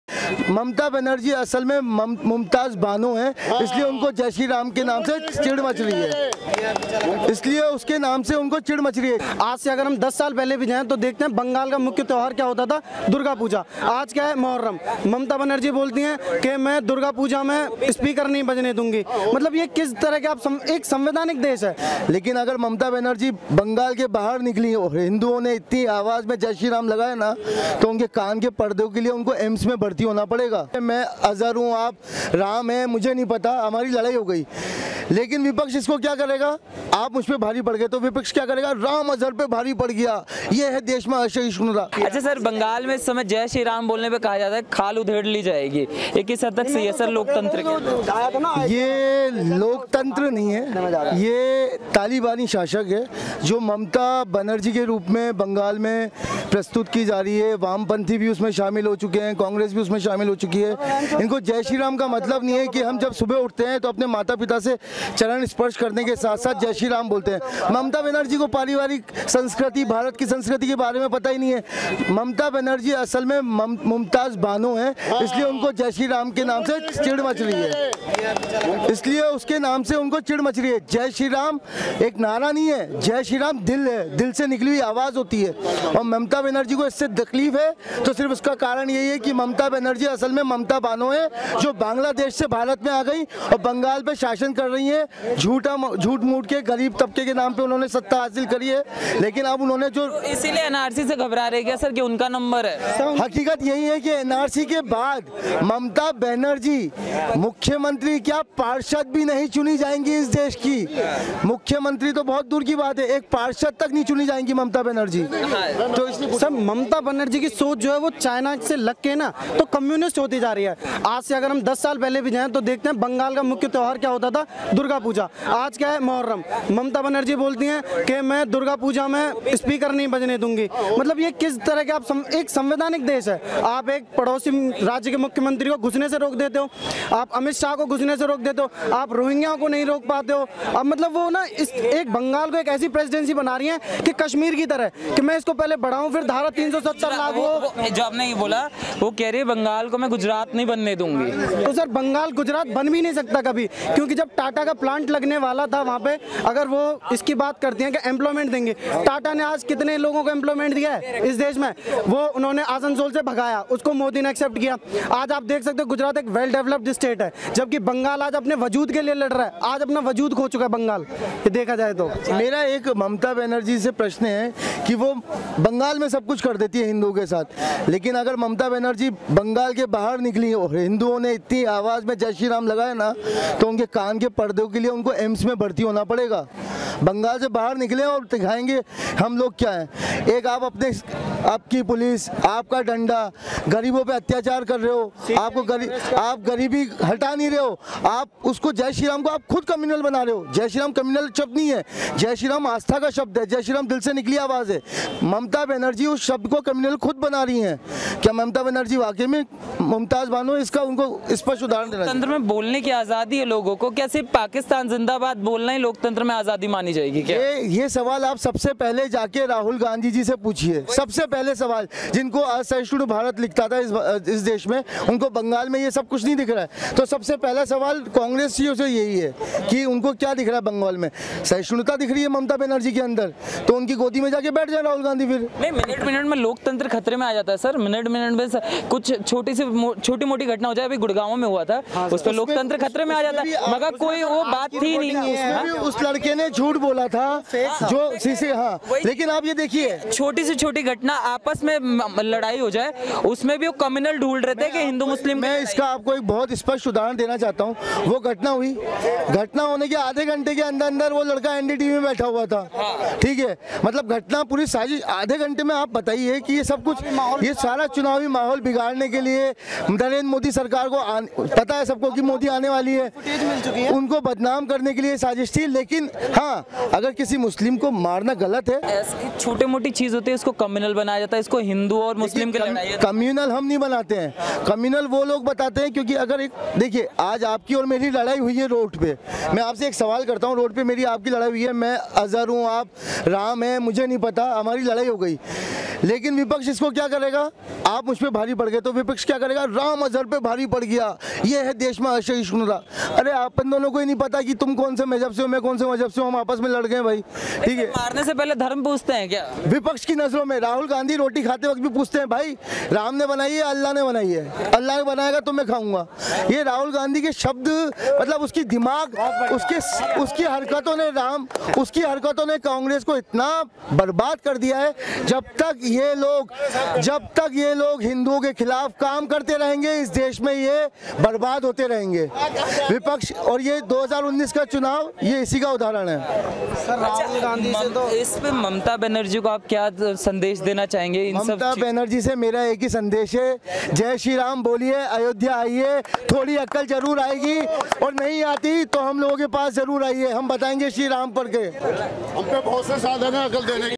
तालिबानी शाशक Mamta Banerjee को इस व्यक्ति ने दी चेतवानी बंगाल से बहार आओ | Public Reaction
Anchor : Jitendra Shukla

वीडियो पसंद आयी हो तो लाइक जरूर करे
हम ऐसे ही जनता की राये आपके सामने लाते है